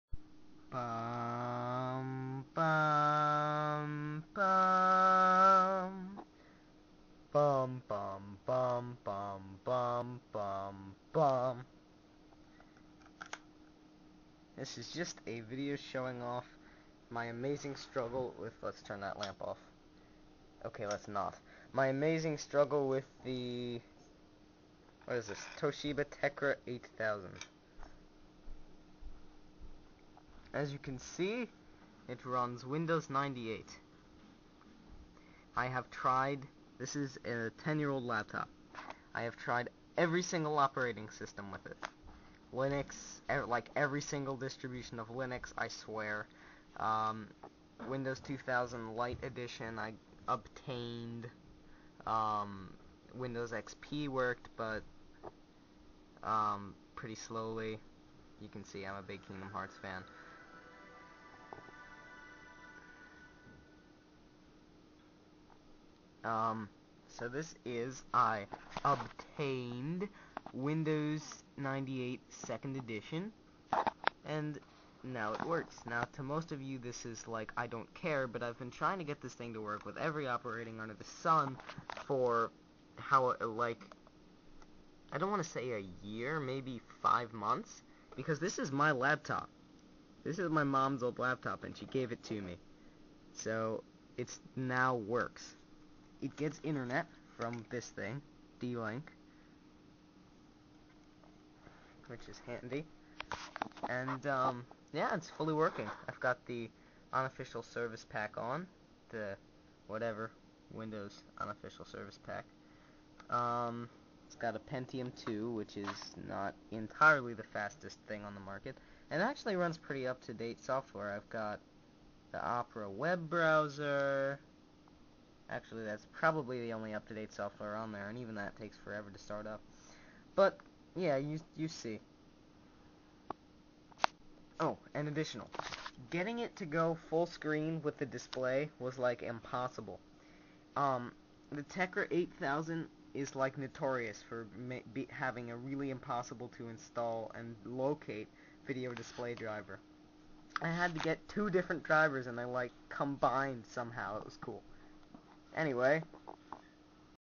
Vlog #1: The 5 month Struggle
Though none of you care, I get bragging rights for reviving a seemingly dead laptop.
The Operating Systems I tried:
Windows Xp (too slow)
Xubuntu(gave me weird errors)
Fluxbuntu(gave me more wierd errors)
Damn Small Linux (Froze when trying to load some drivers. Froze when I told it not to load the drivers)
Puppy Linux (don't quite remember, must have involved weir errors)
Windows 2000 Lite (Installed, but I could only use about 2/3 of the screen and was limited to 16 colors, and the drivers to fix this only worked with '98)
Windows 98 SE (At first did the same thing Windows 2000 did, but I installed a bunch of drivers and it worked fine)